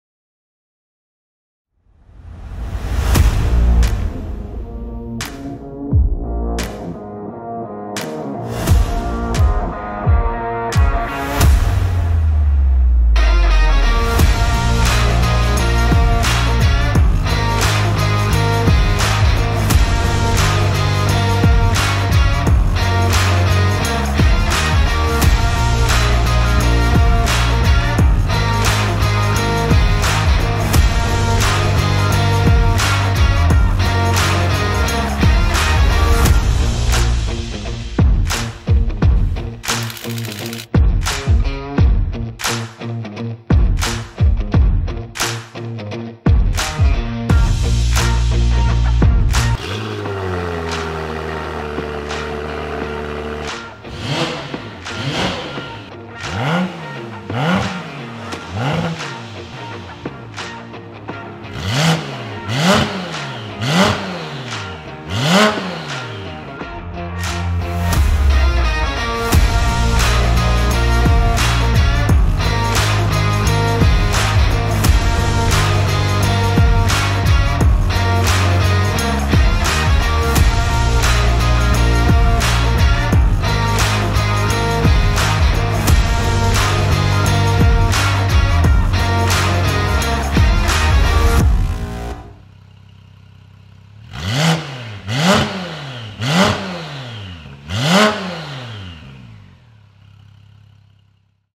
Porsche 991.2 Carrera - Kline Innovation
Porsche 991.2 Carrera - Valvetronic exhaust system available with cat pipes and equal lenght headers.
The system is installed by our friends from Brazil - Mandalá Motorsport.